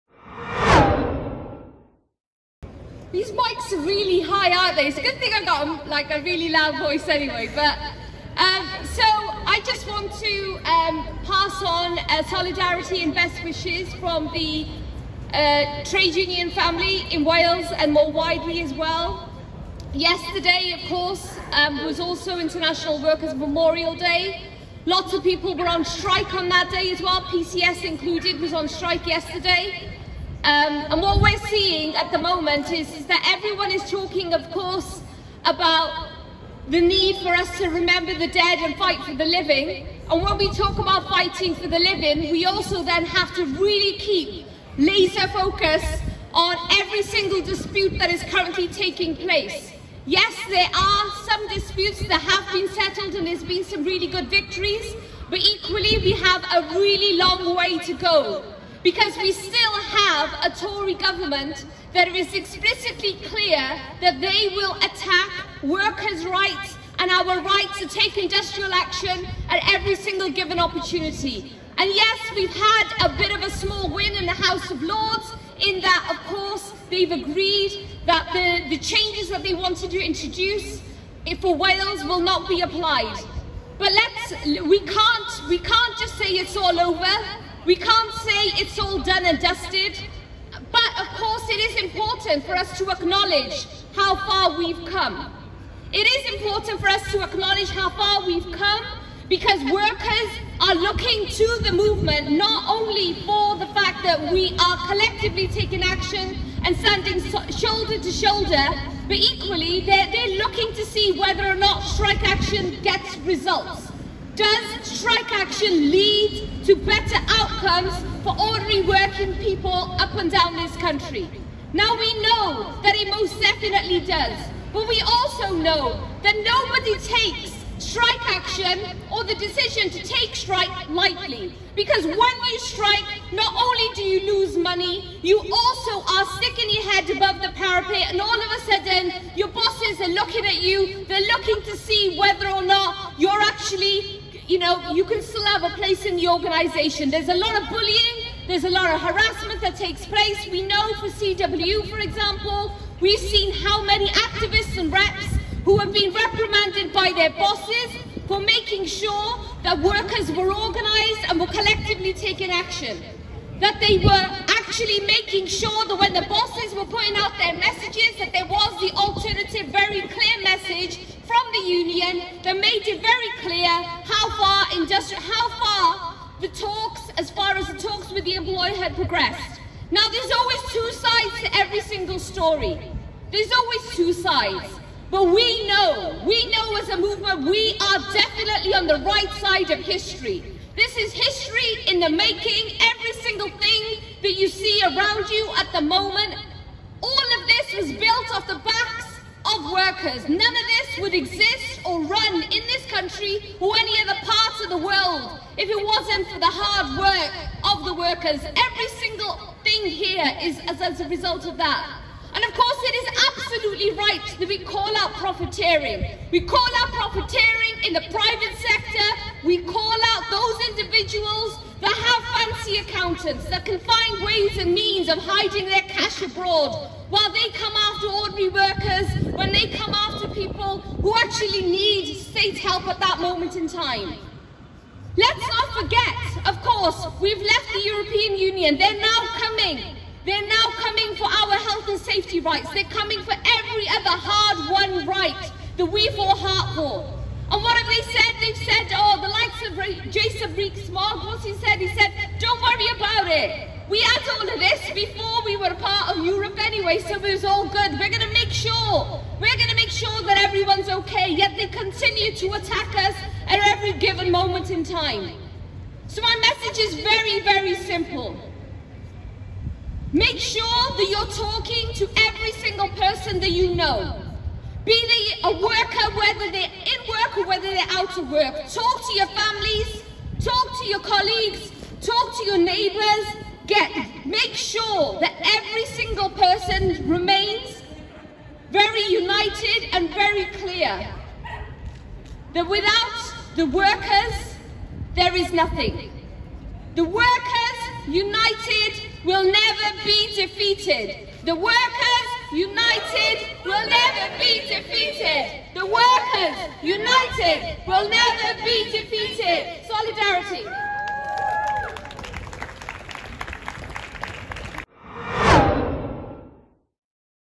Shavanah Taj - Cardiff Annual May Day March & Rally
Shavanah Taj The General Secretary of the Wales TUC has joined Cardiff Annual May Day March & Rally. The event was organized by the Cardiff Trades Council.
Thanks!!!